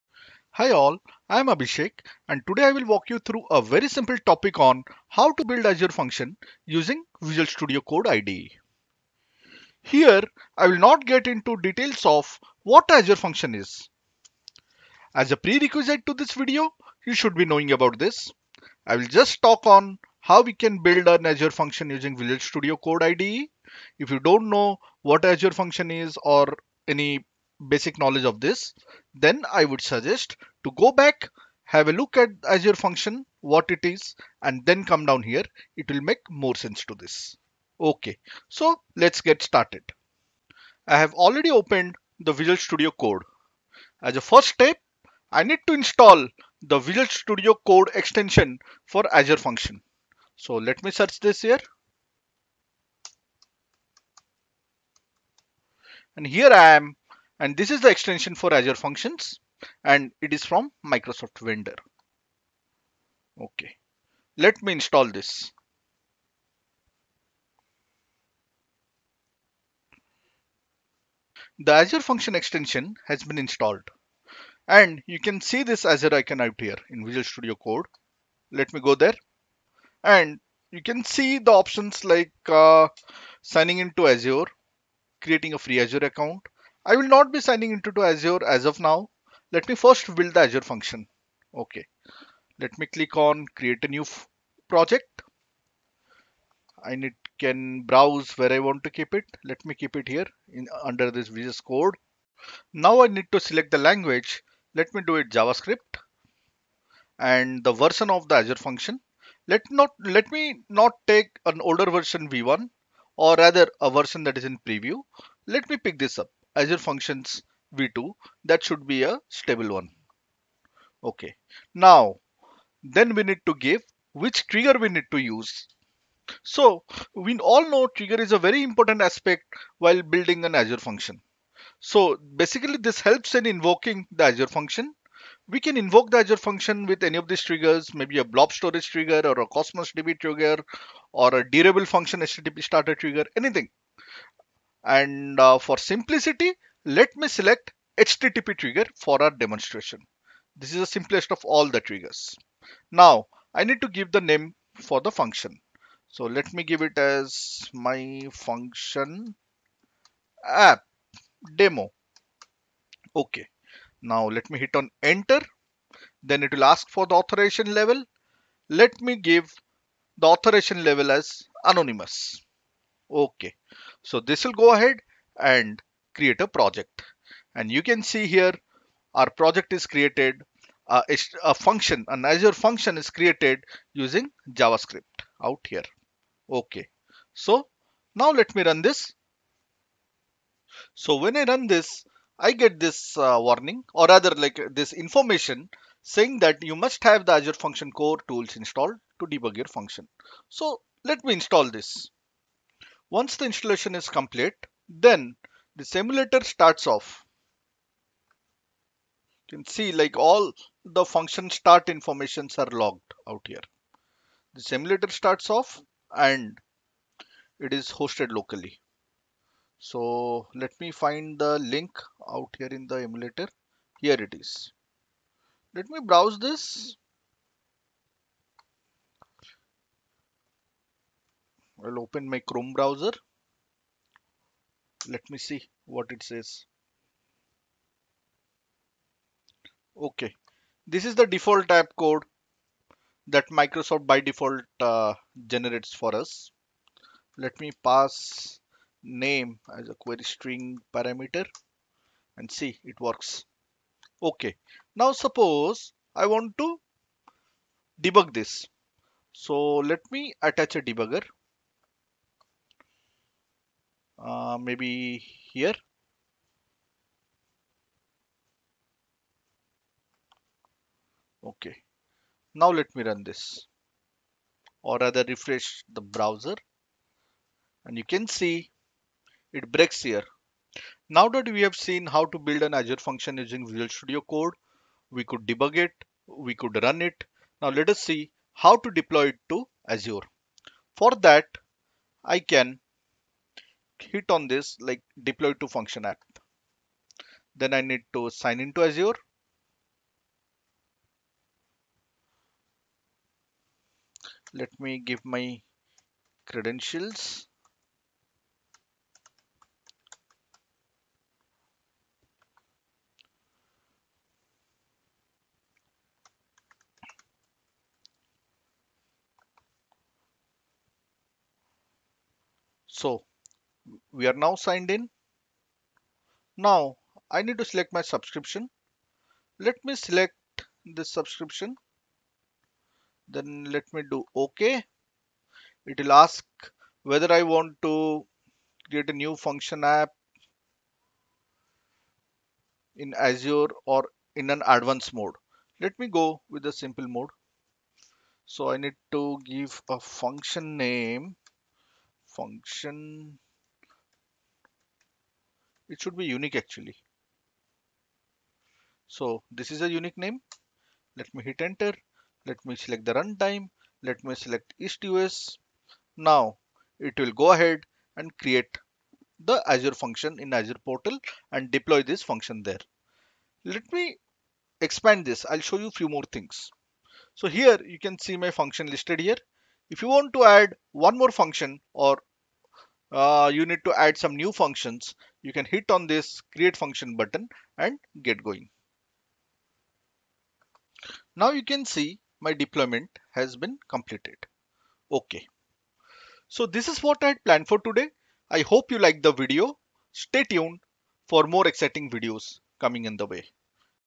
Create Azure Function using Visual Studio Code IDE
This video describes how to create and debug Azure Functions using Visual Studio Code IDE.